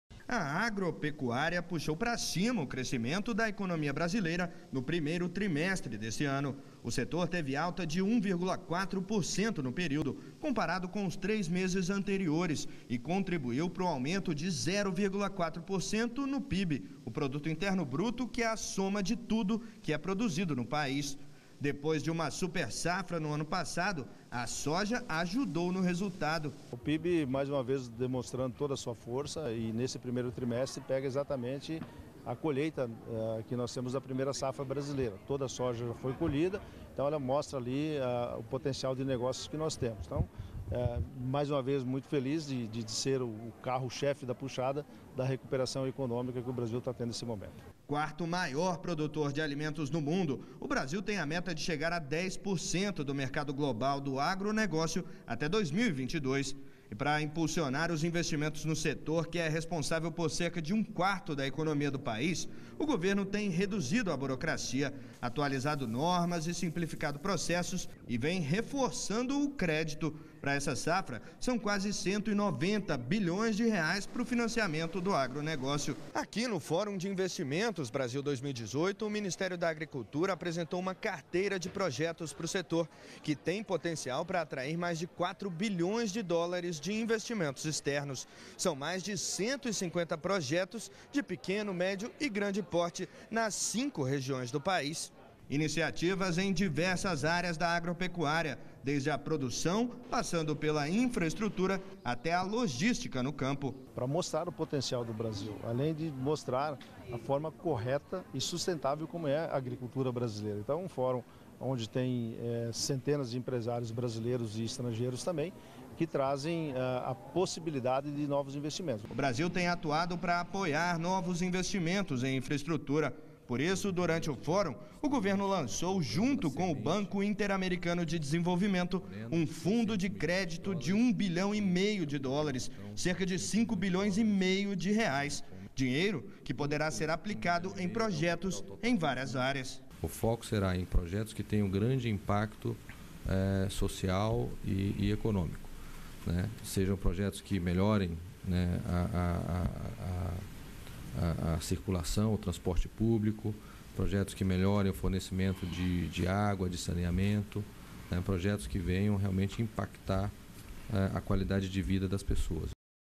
Agronegócio é destaque no panorama de investimentos no Brasil
REPÓRTER NBR – 30.05.18:  Os negócios do campo ganharam destaque nos debates do Fórum de Investimentos Brasil  2018, realizado em São Paulo. O setor agropecuário tem sido destaque no crescimento do Produto Interno Bruto, o PIB do país.